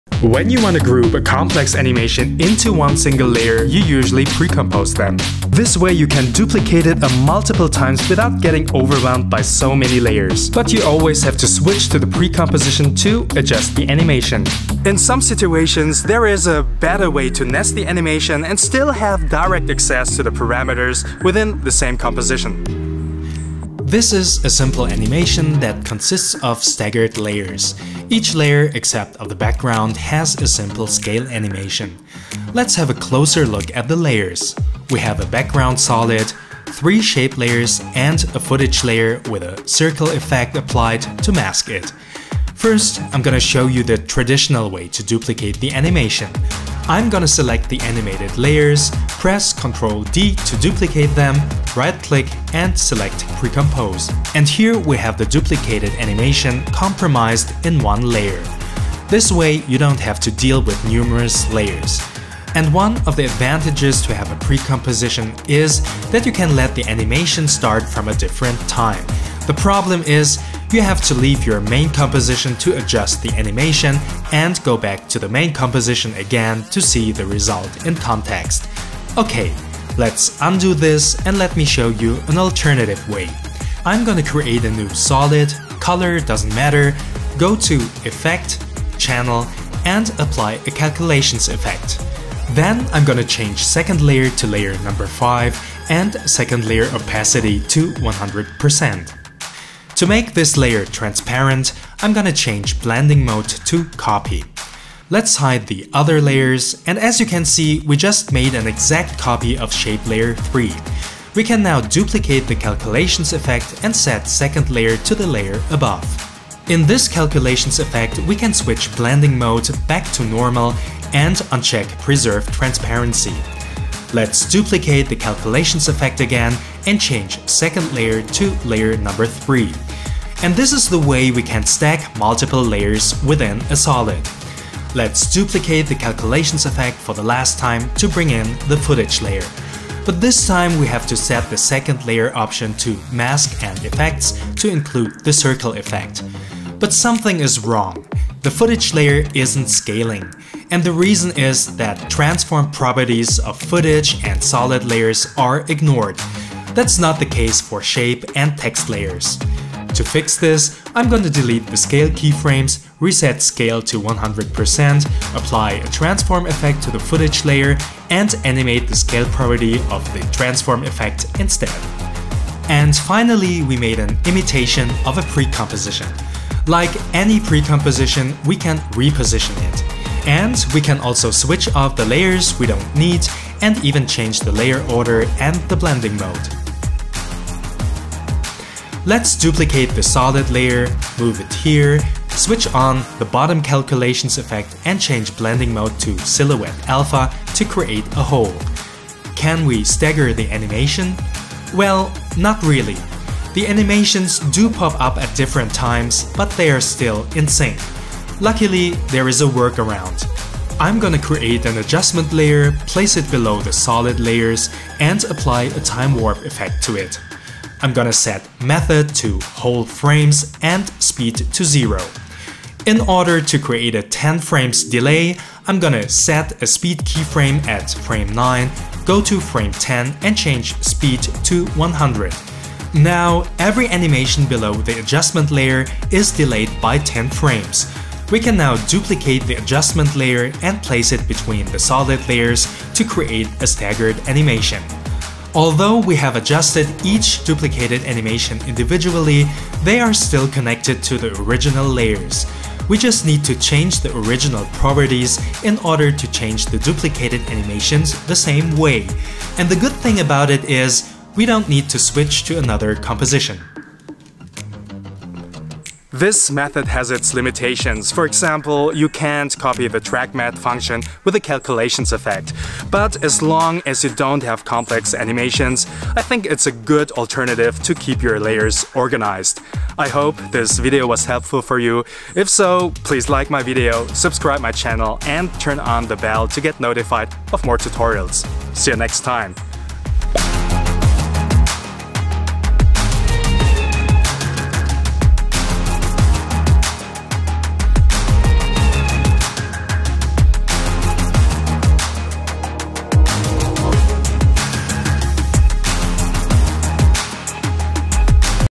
You DON'T NEED to PRE-COMPOSE | Another Way to Group Layers in After Effects | Quick Tip Tutorial
When you want to organize & group layers in After Effects you usually use precompositions.

In this Quick Tip After Effects Tutorial I'm going to show you another way to group layers.

There is one useful and underrated channel effect to accomplish this.

By the way, I used already used this technique in some of my tutorials: